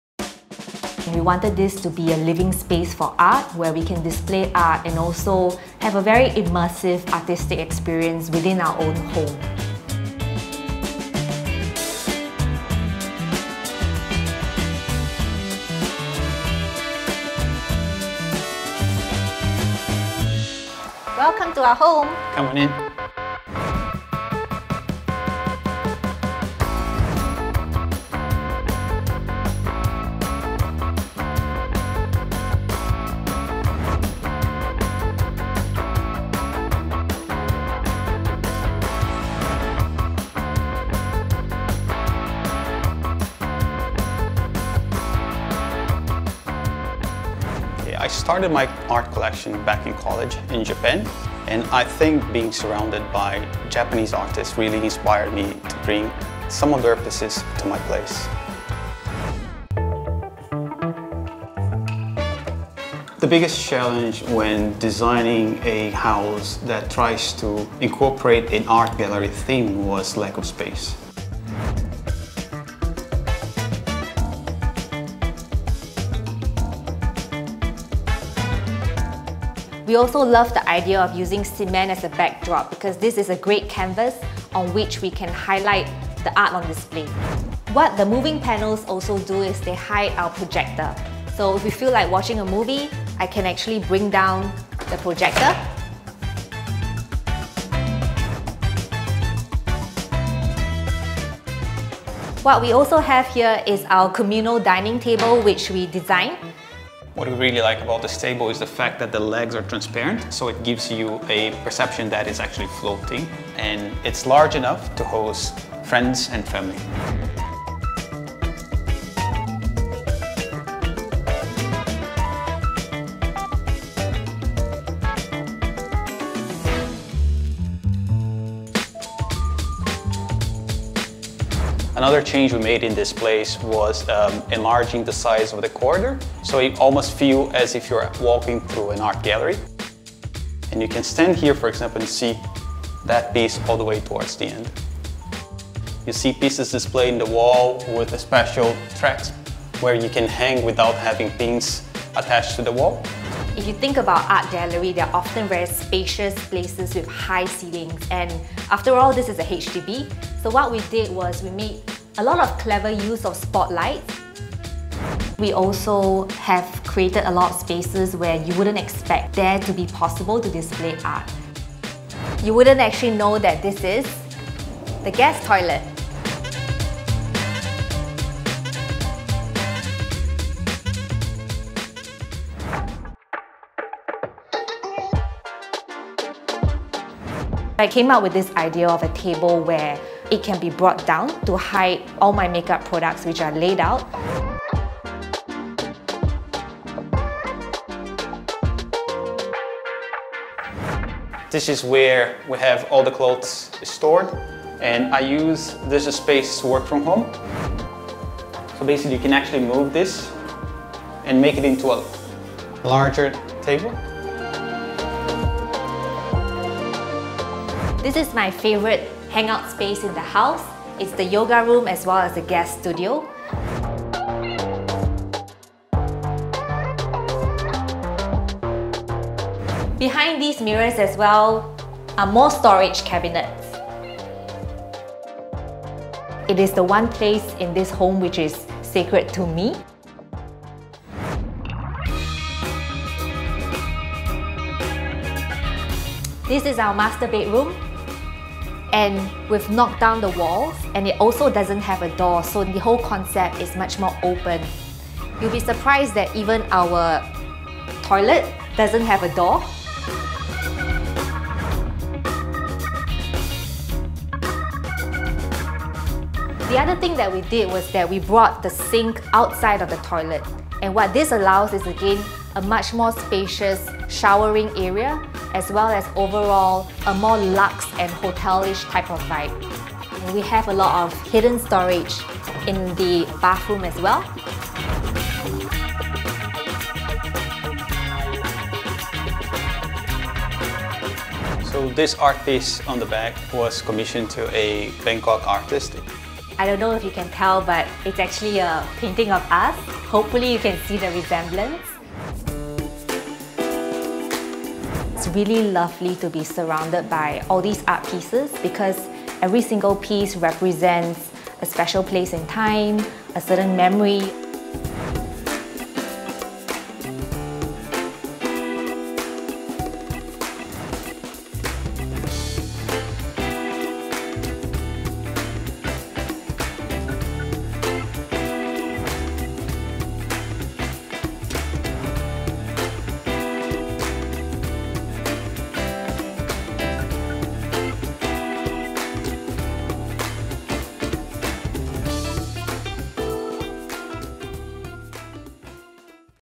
Making Room: An HDB flat with an art gallery and a yoga room | CNA Lifestyle
CNA Lifestyle’s Making Room series looks at small homes with big transformations. This week, we check out an apartment that features clever tricks for displaying art pieces.